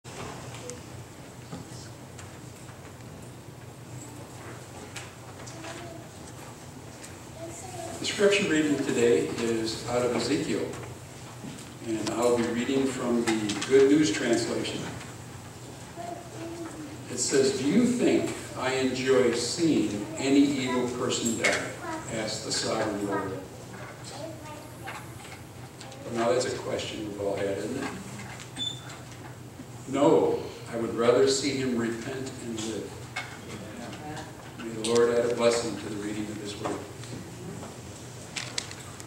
NSBSDA - Scripture Reading - August 10, 2019
Presented By: Marty
Scripture: Ezekiel 18:23
New Smyrna Beach Seventh Day Adventist Church